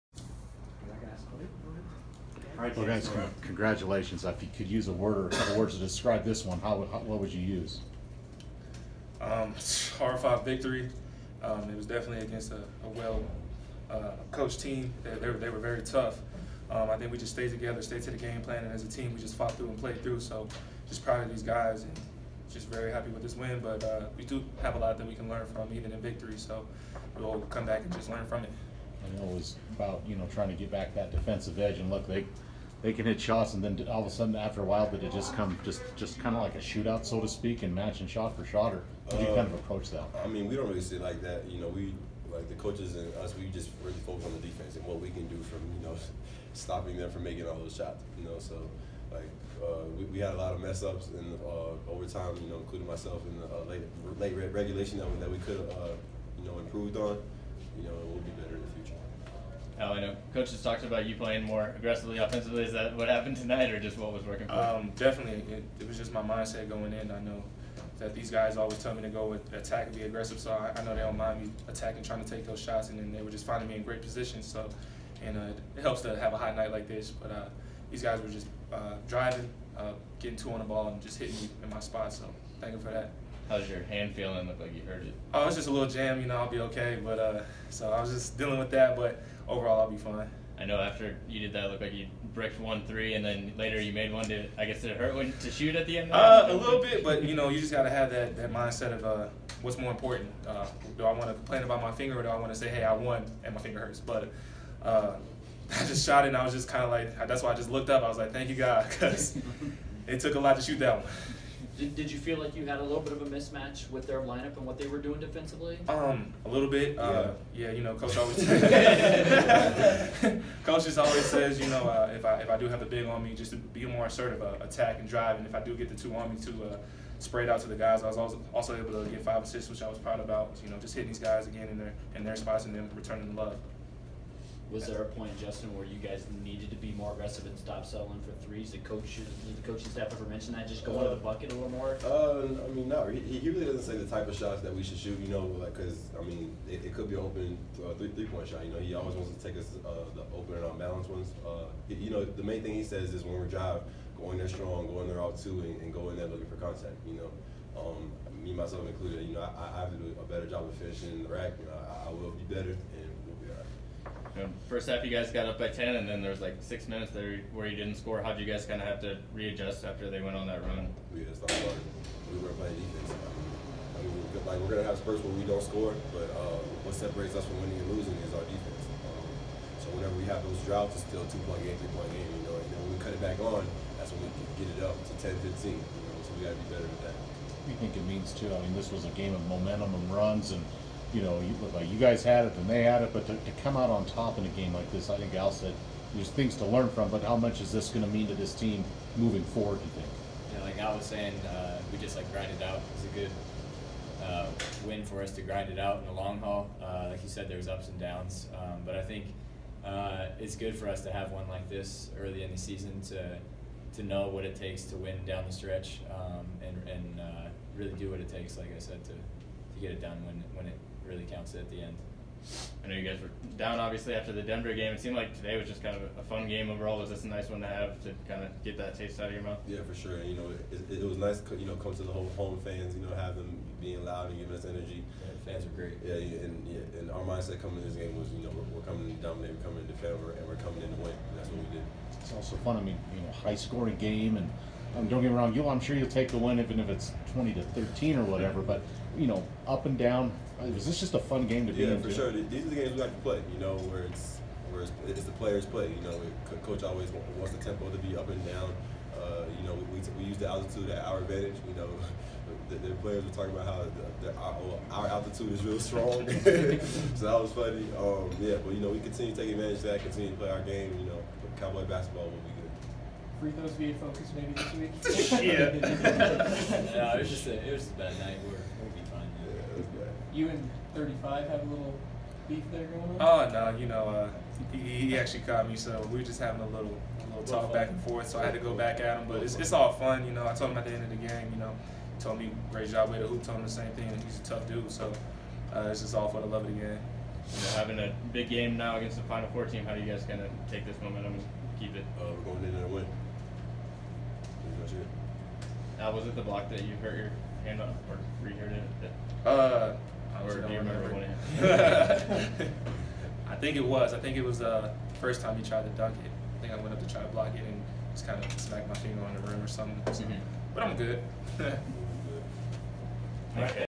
Wyoming players -- Drake postgame
From left to right, senior forward Hayden Dalton, junior guard Justin James and senior forward Alan Herndon talk to the media after the Cowboys' 96-89 double-overtime victory over Drake on Saturday, Dec. 2, 2017 at the Arena-Auditorium in Laramie, Wyoming. All three players finished with double-doubles. Video by Robert Gagliardi/WyoSports